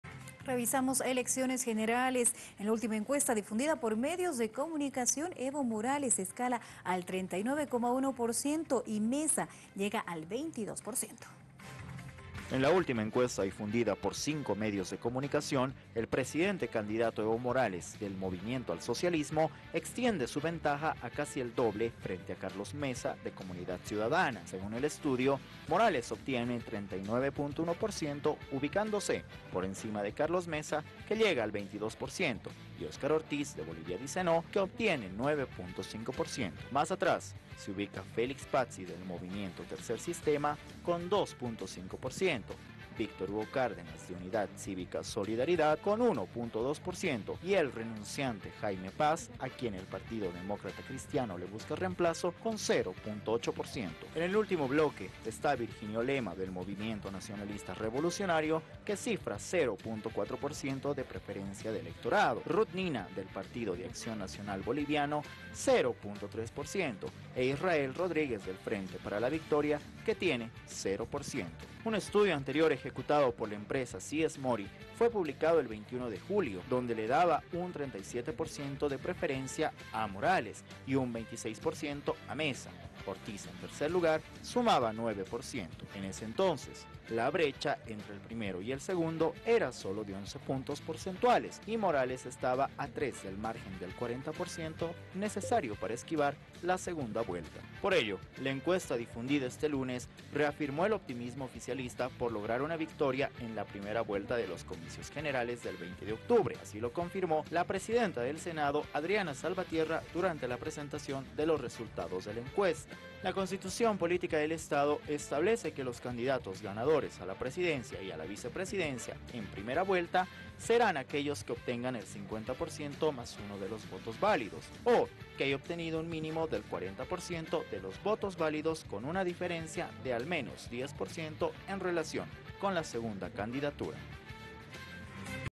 Encuesta revela que Morales sube el 39,1%; Mesa cae al 22% y avizoran un triunfo sin segunda vuelta
La encuesta publicada por Vías Bolivia posiciona al candidato presidencial por el MAS, Evo Morales, en el 1er puesto, respecto a la preferencia de partidos, con un 39,1 por ciento, logrando una diferencia de 17 por ciento con el segundo lugar, ocupado por el candidato por el Comunidad Ciudadana (CC), Carlos Mesa, quien se encuentra con un 22 por ciento y la Alianza Bolivia Dice No, de Óscar Ortiz, con el nueve por ciento.